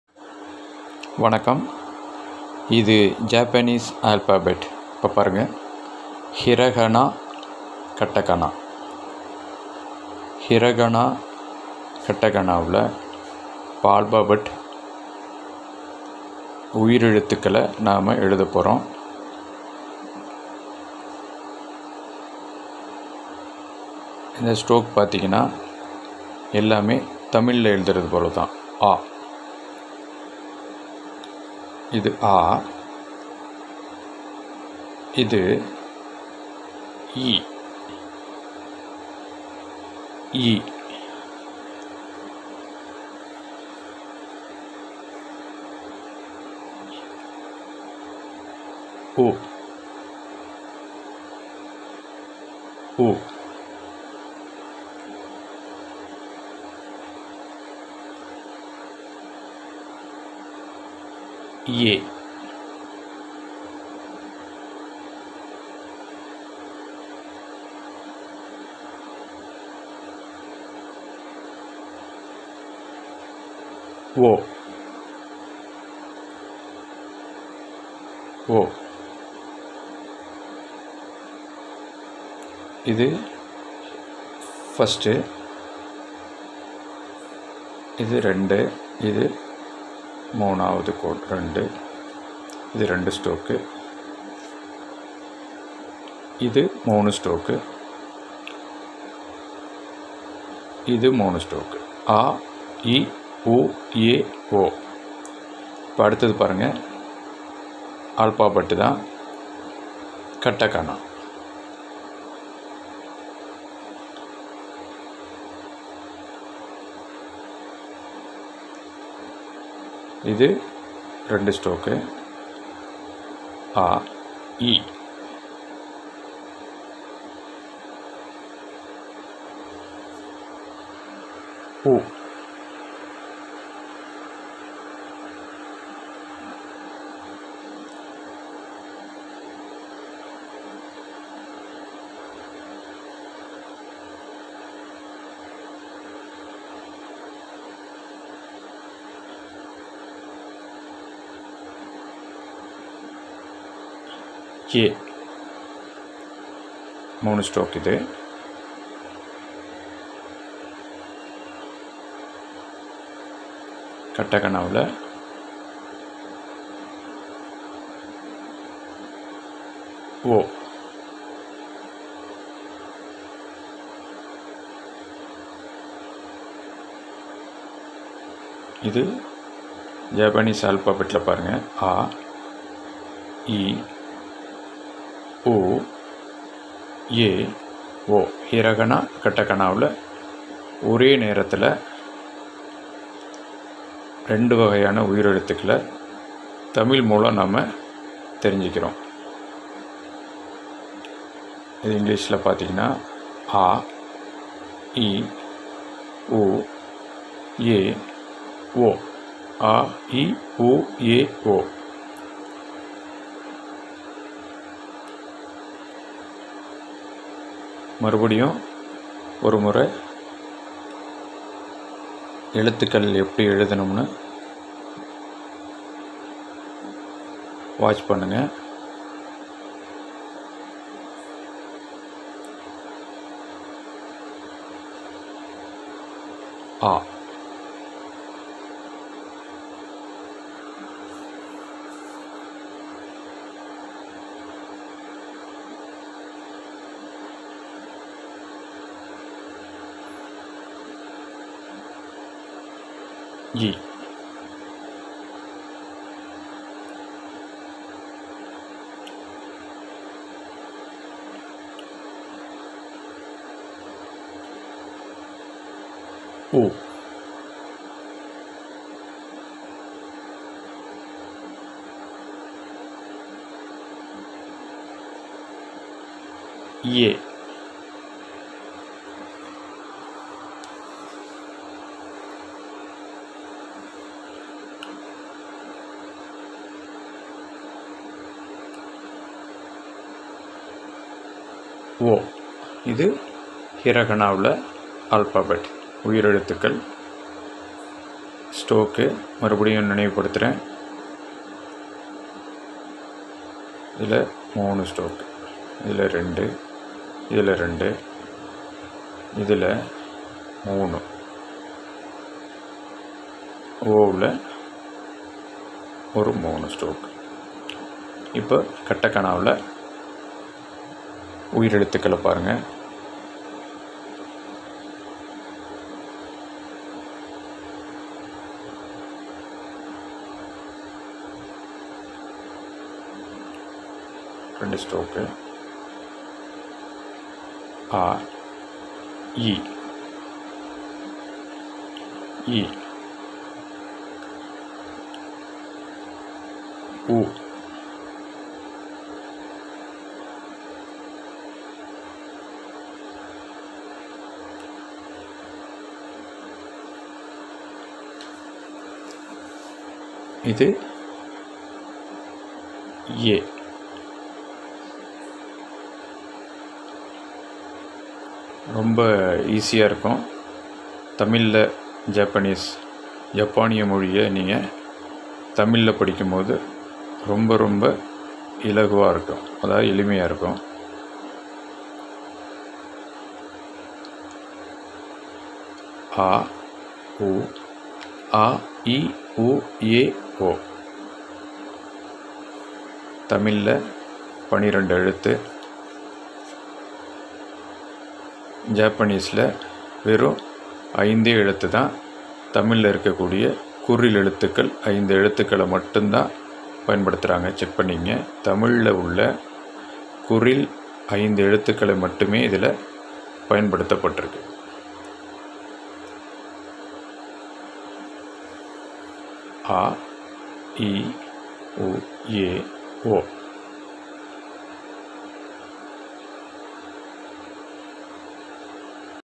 Learn Japanese through Tamil - lesson 1 - Hiragana & katagana alphabet
Learn Japanese through Tamil - video 1 - Hiragana & katagana alphabet
Japanese speaking and learning in tamil